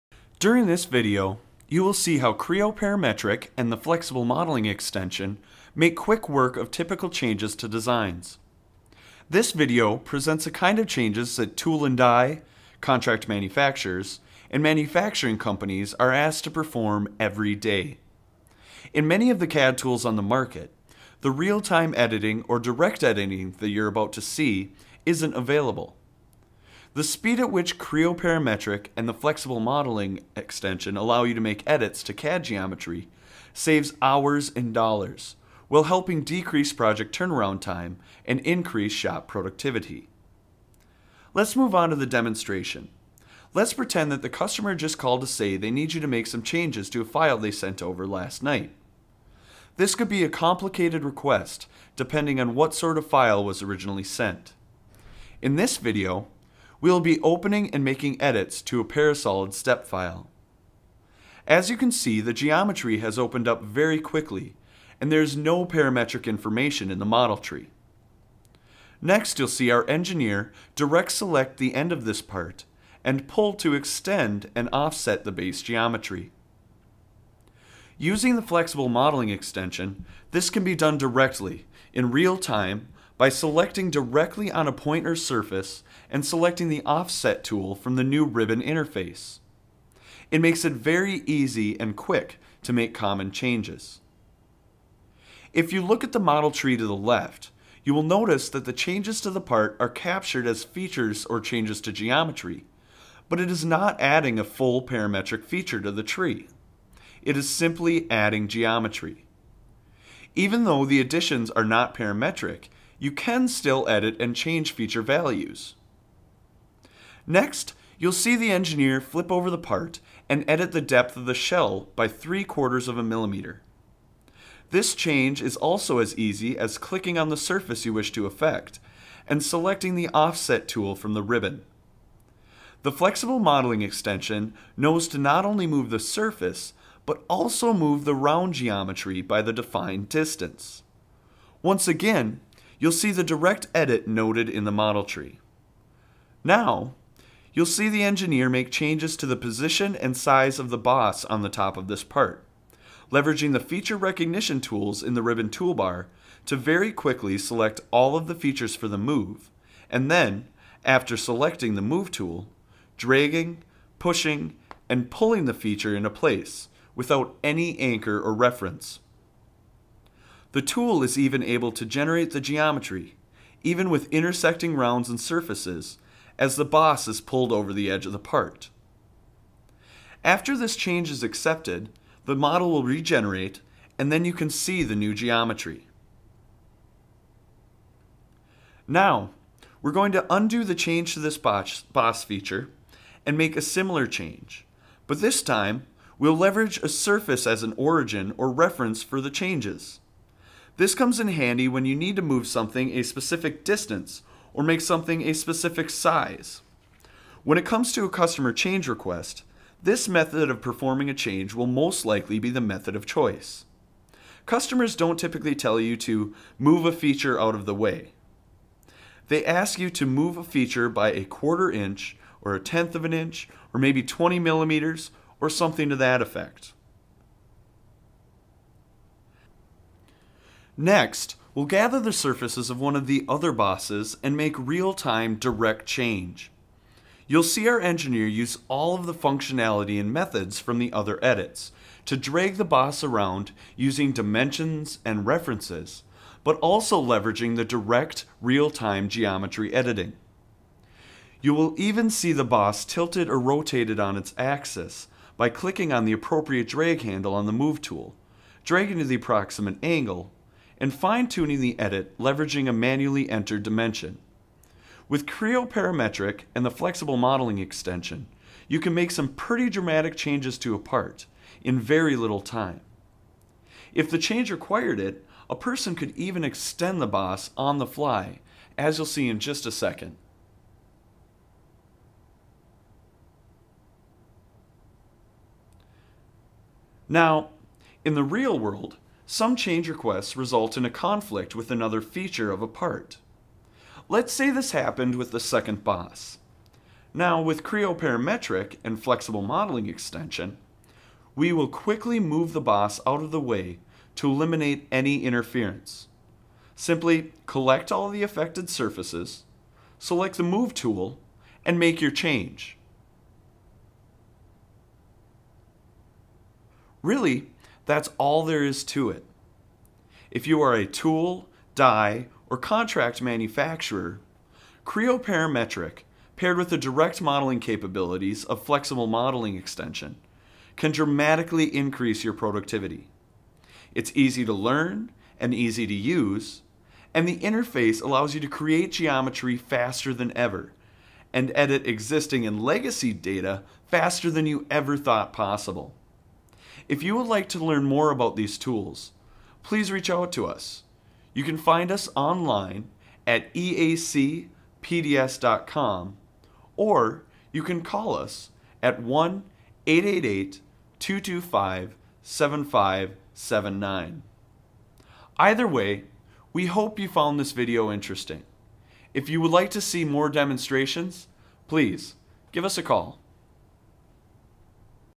Making a design change in Creo Parametric with the Flexible Modeling Extension
This 6 minute video shows a demonstration of how easy it is to make a change to an existing 3D CAD part or para-solid file when working in Creo Parametric with the Flexible Modeling Extension.